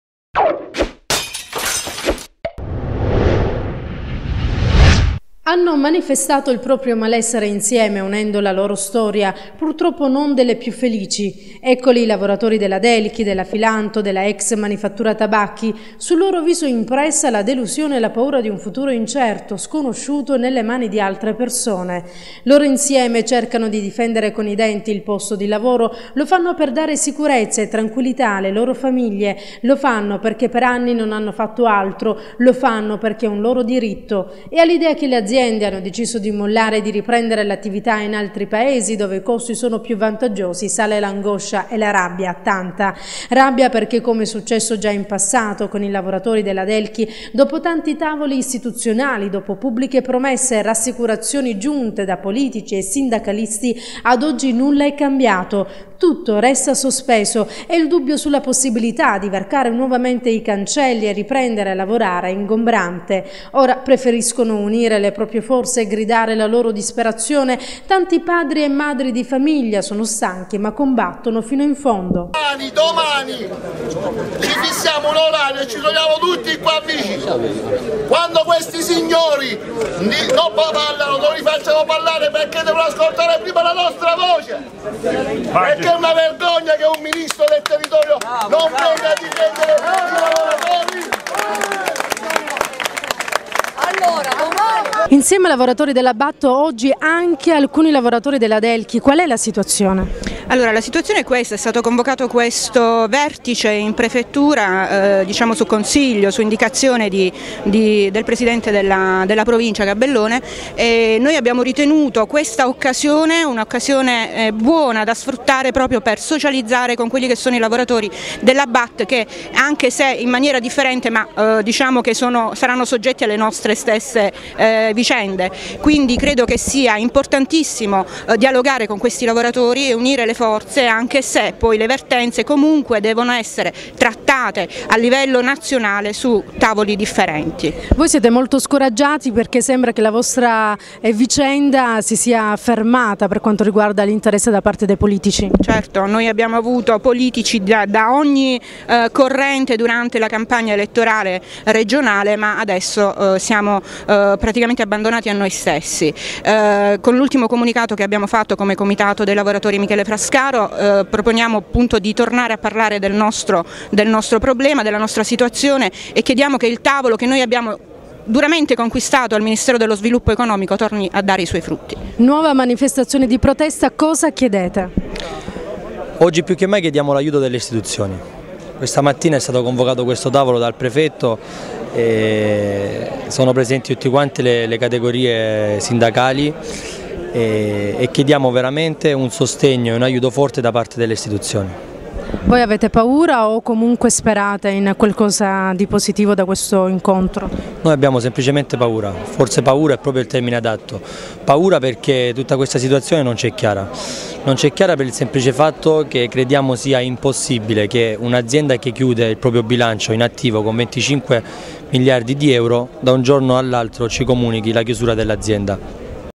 Lavoratori Adelchi, Filanto e Bat: insieme per difendere il proprio lavoro
Hanno manifestato presso la Prefettura di Lecce mentre su si teneva un vertice. I lavoratori Adelchi, Filanto e della ex Manifattura Tabacchi hanno unito la loro voce per gridare tutta la loro amarezza e delusione. Vogliono certezza per il futuro, sono stanchi di tavoli e promesse al vuoto: dopo tante parole pretendono fatti.